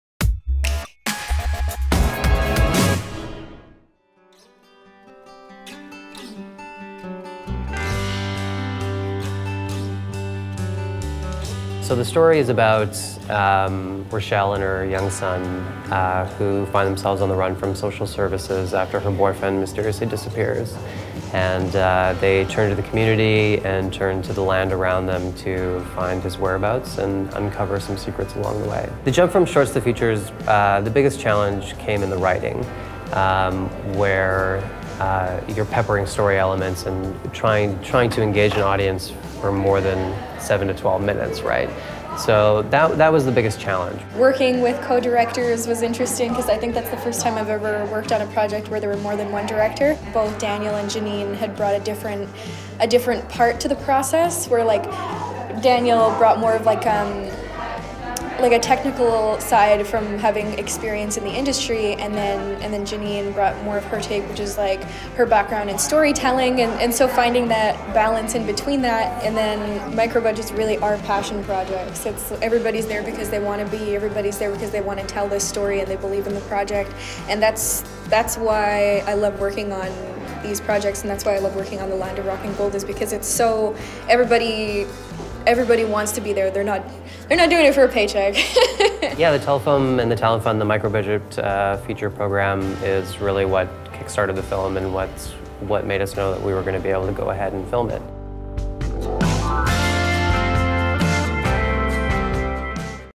The Land of Rock and Gold Interview
Following its World Premiere at imagineNATIVE last month, the made-in-Saskatchewan mystery drama The Land of Rock and Gold is now set to screen at Regina's Artesian this Friday. Check out our interview with co-director Daniel Redenbach and actor Devery Jacobs to know more about this film financed through our Microbudget Program.